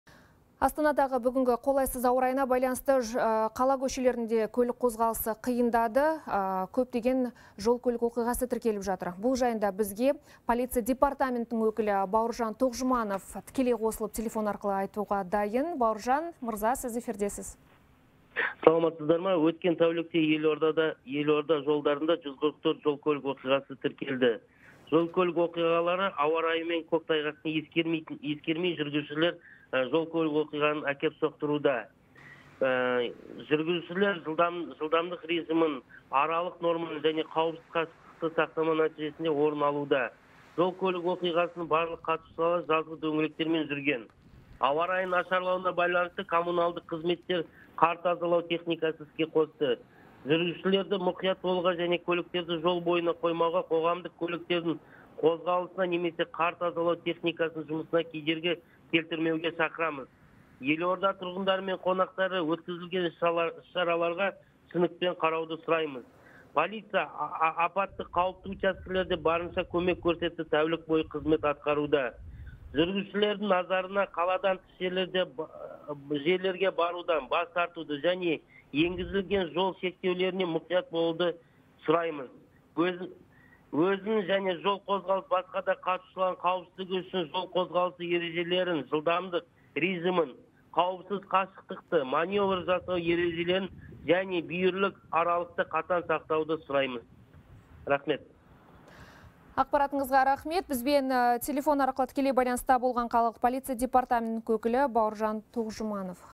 Астана тұрғындары автобусты сағаттап күтуге мәжбүр. LIVE
Астанада кешелі бері жауған қар салдарынан көлік қозғалысы қиындады.